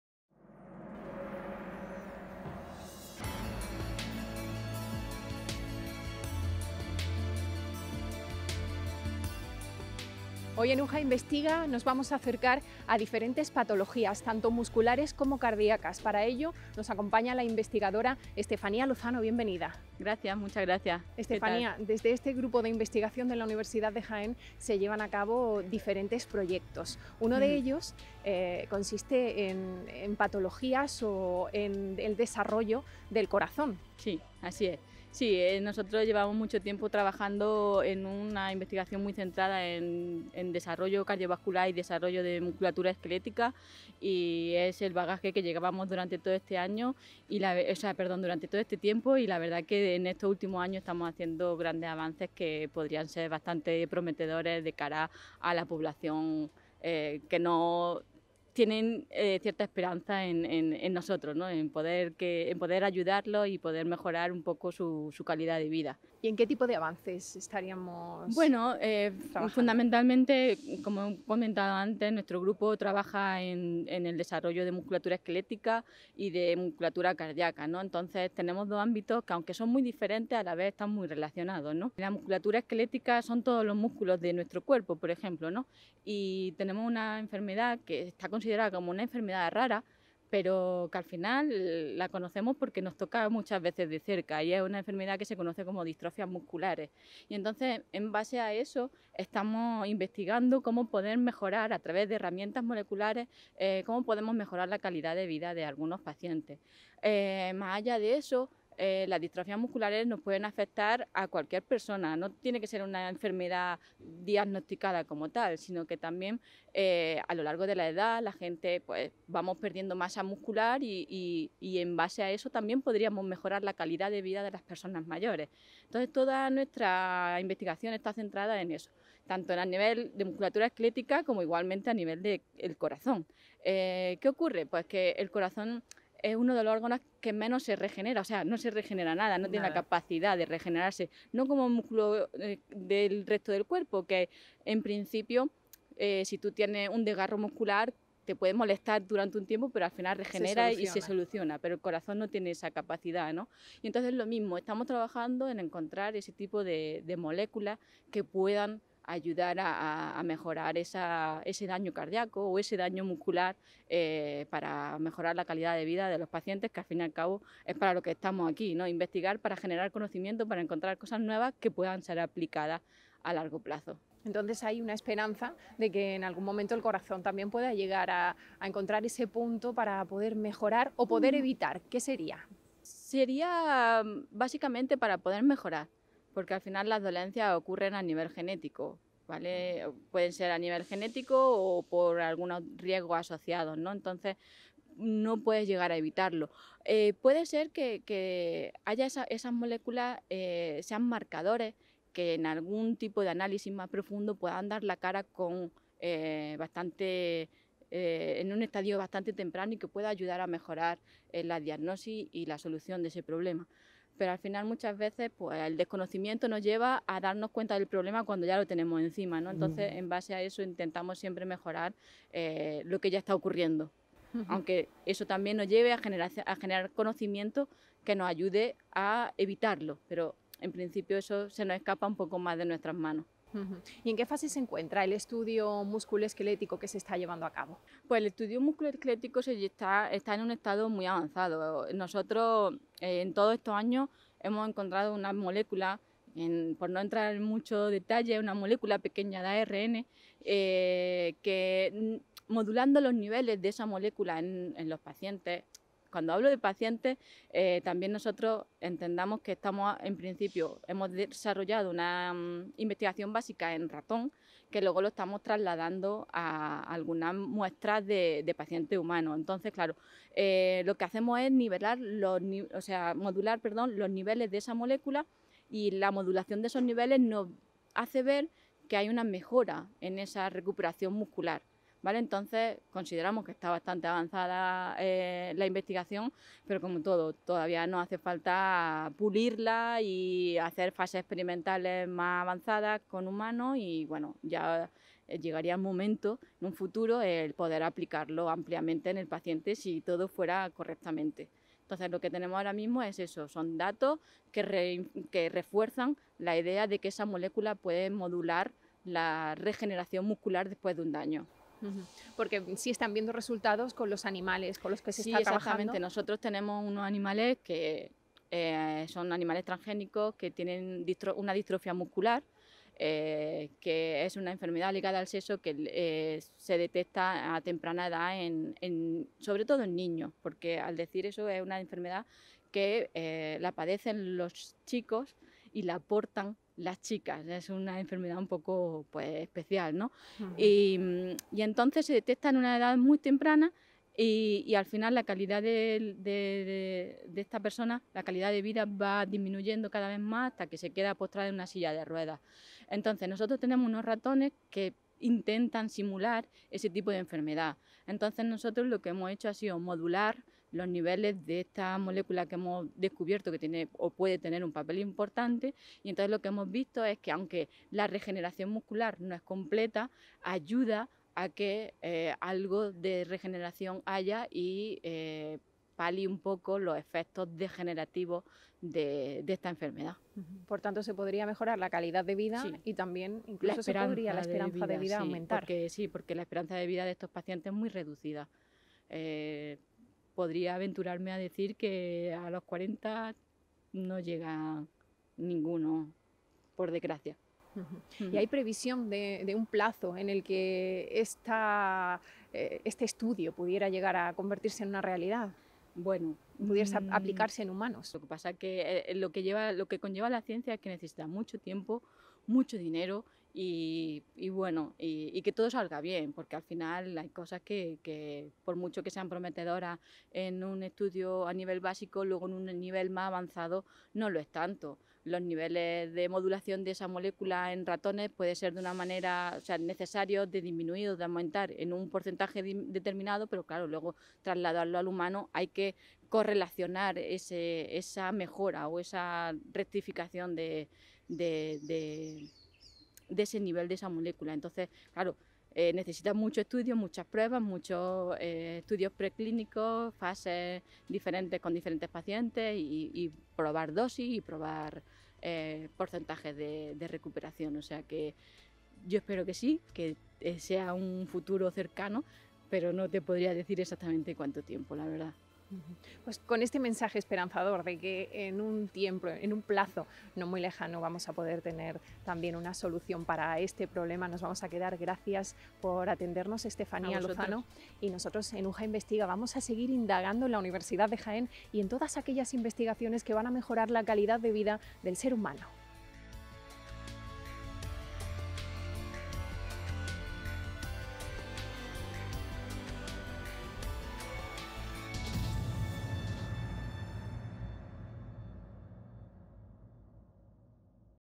UJA INVESTIGA Desarrollo cardiovascular y musculoesquelético
Hoy en UJA Investiga, la investigadora de la Universidad de Jaén Estefanía Lozano nos habla sobre un estudio que pretende poder mejorar la calidad de vida de pacientes con distrofia muscular, una enfermedad que puede afectar a cualquier persona y en la que se pierde masa muscular, tanto a nivel general como en el corazón.